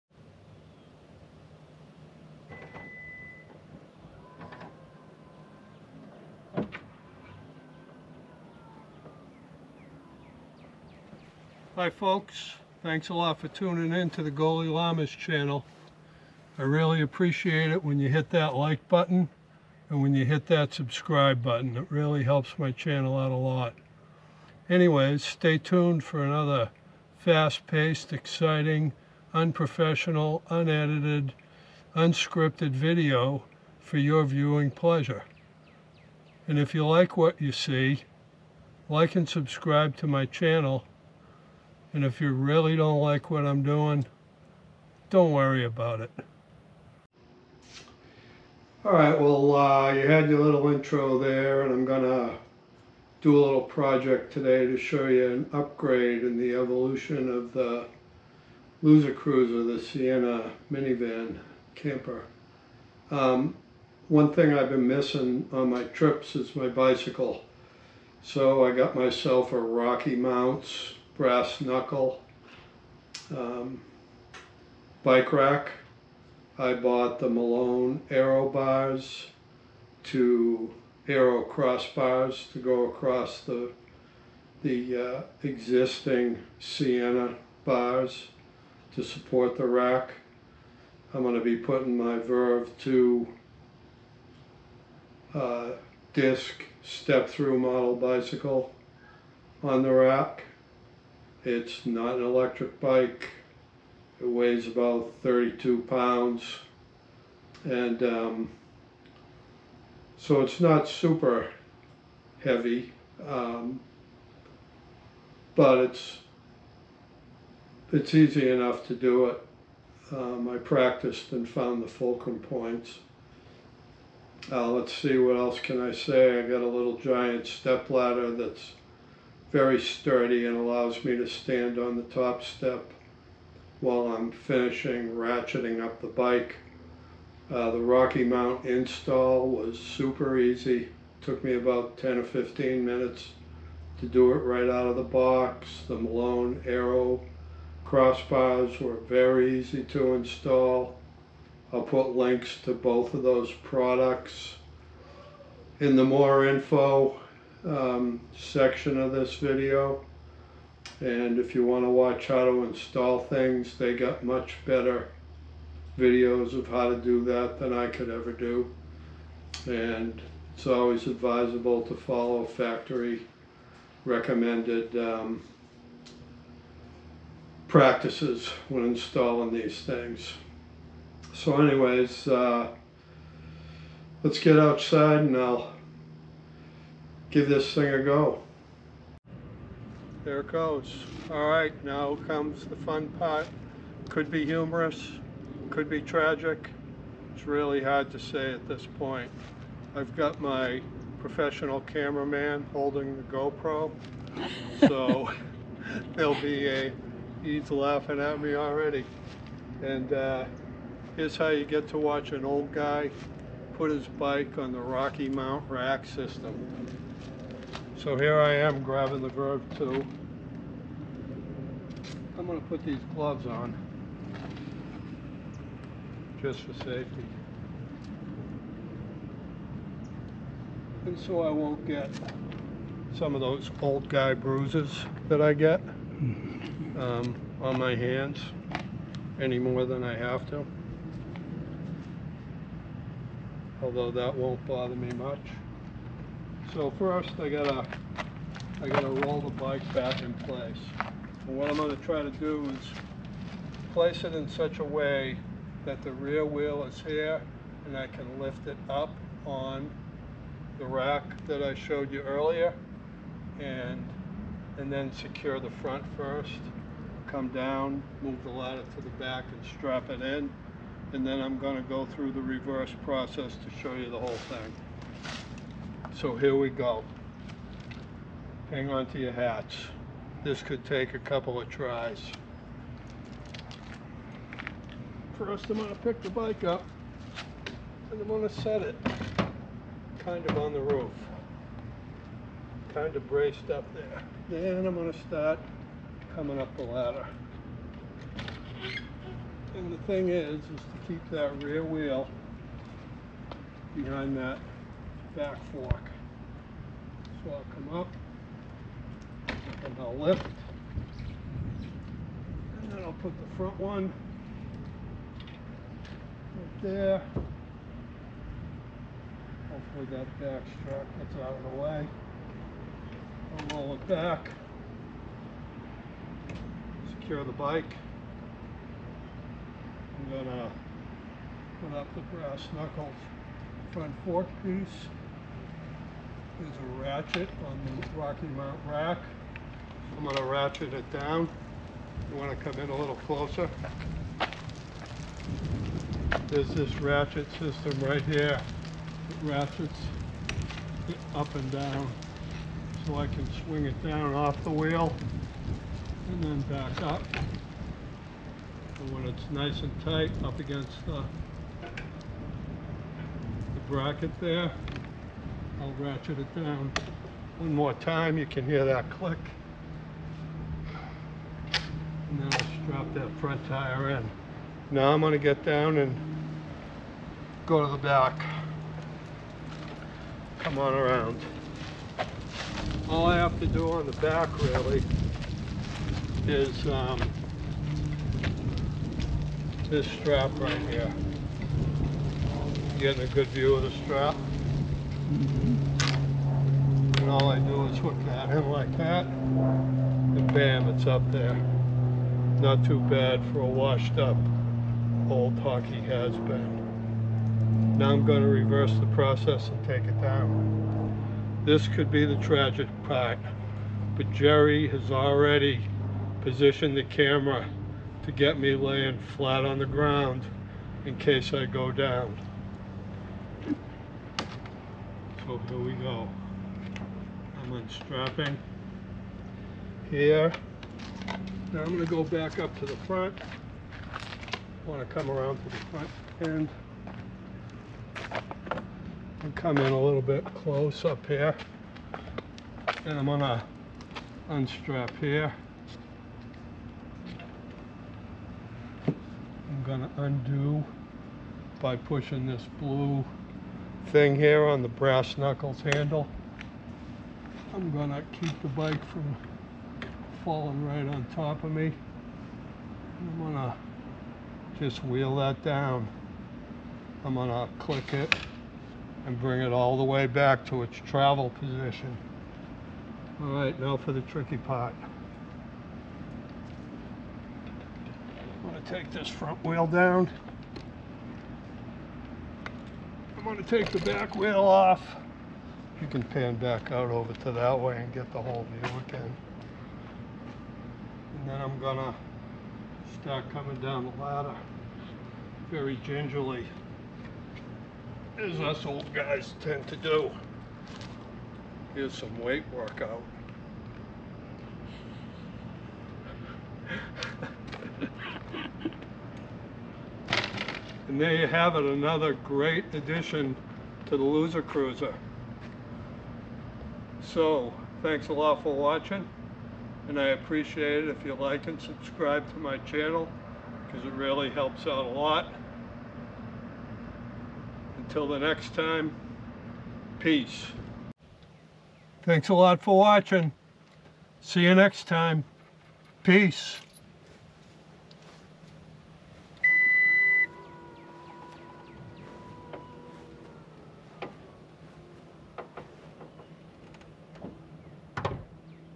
Sienna Camper Evolution - RockyMounts Brass Knuckles Bike Rack
Had to do something about taking my bike along in my travels.
Bought a couple of items to upgrade the Loser Cruiser. Expanding the capabilities as I go.
Both the crossbars and the bike rack were very easy to install. Even for the Ancient Goalie Lama!

I'm just showing the practical application of getting the bike up on the roof!
9'6" height when the bike is up, so I'll need to be aware of that.

Otherwise, it's fairly simple...As you will see.

The learning curve continues with CyberLink PowerDirector 365.

Little Giant Ladders, Sure Step, 3-Step, Double-Sided Step Stool, Fiberglass-